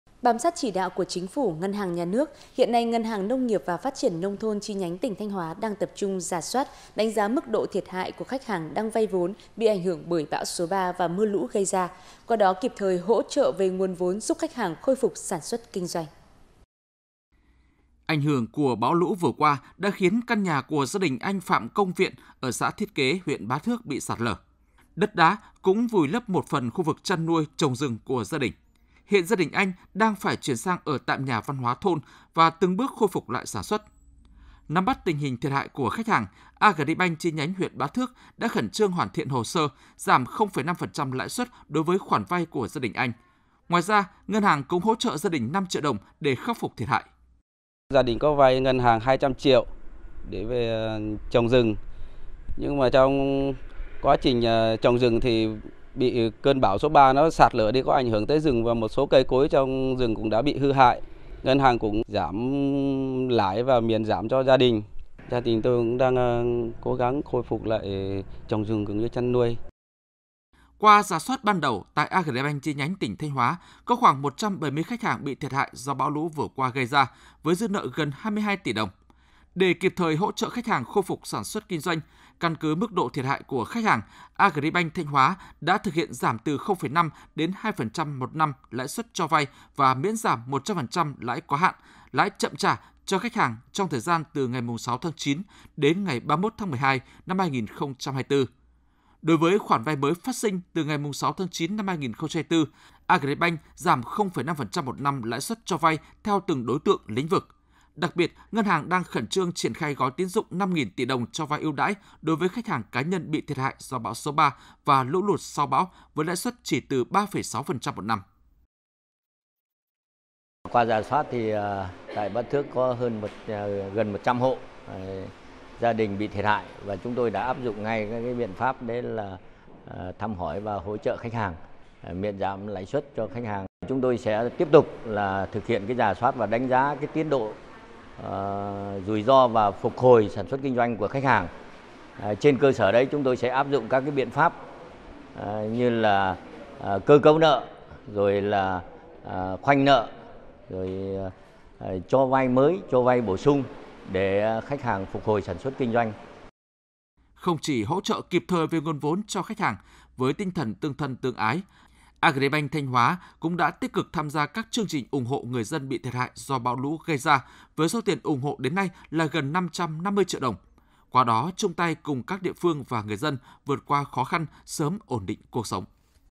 Agribank Thanh Hoá hỗ trợ khách hàng bị thiệt hại do bão lũ khôi phục sản xuất kinh doanh
Bám sát chỉ đạo của Chính phủ, ngân hàng Nhà nước, hiện nay Ngân hàng Nông nghiệp và Phát triển nông thôn chi nhánh tỉnh Thanh Hoá đang tập trung rà soát, đánh giá mức độ thiệt hại của khách hàng đang vay vốn bị ảnh hưởng bởi bão số 3 và mưa lũ gây ra. Qua đó kịp thời hỗ trợ về nguồn vốn giúp khách hàng khôi phục sản xuất kinh doanh.